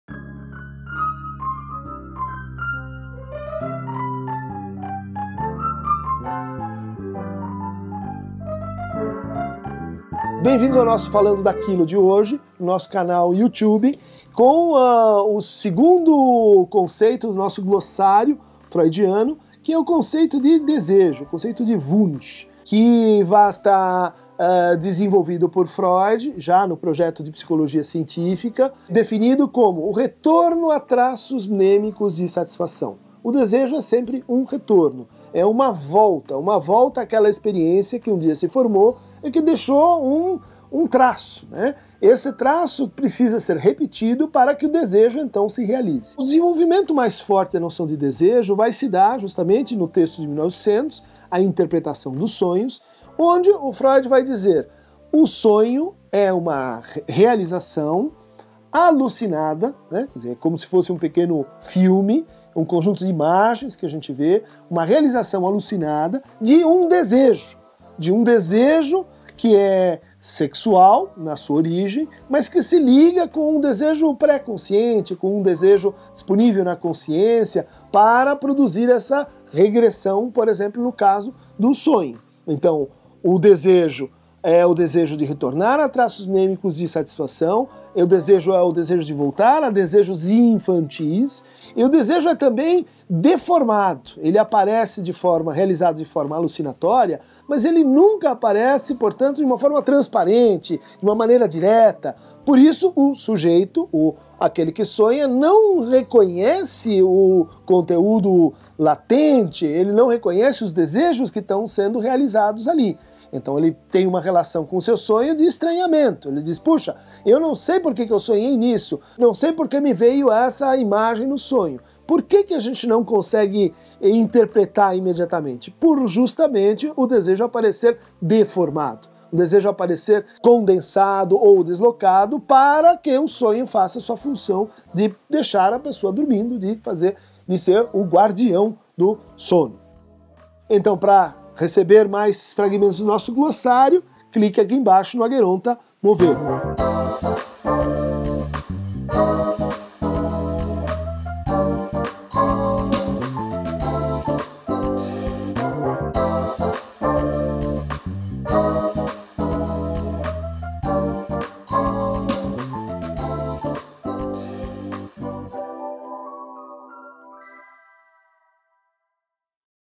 O desejo - Glossário Freud | Christian Dunker | Falando daquilo 9
O novo quadro "Falando daquilo" vai mais devagar! Em uma conversa mais simples, Christian introduz conceitos psicanalíticos aos poucos, sem pressa - de forma mais acessível e voltada para quem quer fazer parte da conversa do Falando nIsso, mas que não tem tanta familiaridade com a área (como é o caso da produção).

Entre dezembro de 2017 e janeiro de 2018, o Falando Daquilo terá um especial Glossário dos conceitos de Freud, com 18 vídeos curtos dedicados a esclarecer várias das ideias de Sigmund Freud.

As perguntas, sugestões e reclamações são sempre bem-vindos na seção de comentários - ou Acheronta movebo, para os frequentadores.

Captação e tratamento de som e vídeo, iluminação, direção e edição são feitas por lucas buli

Motion e colagens feitas por Marília

A trilha sonora é de autoria de Thommaz Kauffmann

2017